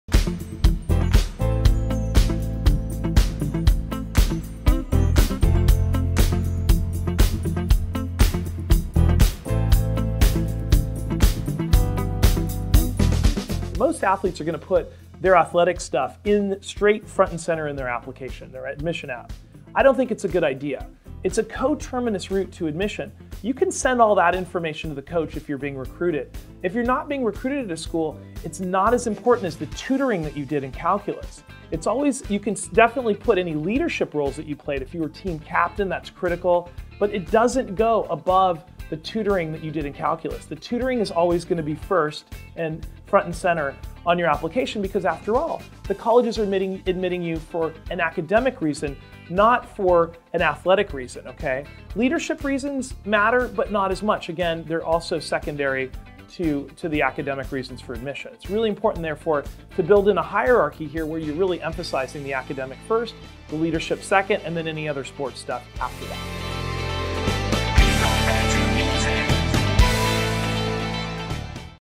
Athletes : How to Position Yourselves : Video
College Match Q&A: Episode 14. College Match Founder David Montesano answers the question "What is the best way for an athlete to represent himself or herself on an application?"

Check back weekly for more of your questions answered, and if you have any questions you would like us to answer, please email .

For more information on College Match US, please visit our or check out